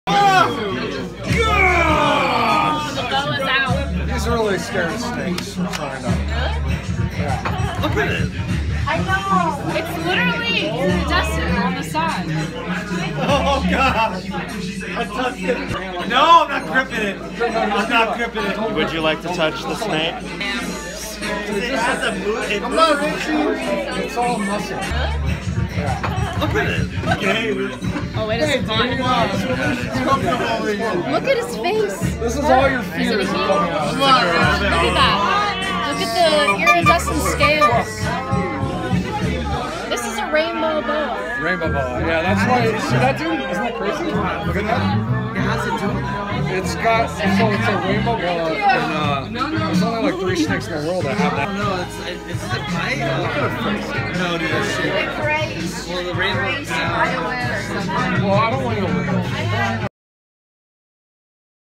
Boa Snake at Nate’s Clown Party!!
Short clip of King Bee’s pet Rainbow Boa Constrictor and me snuggling on Halloween!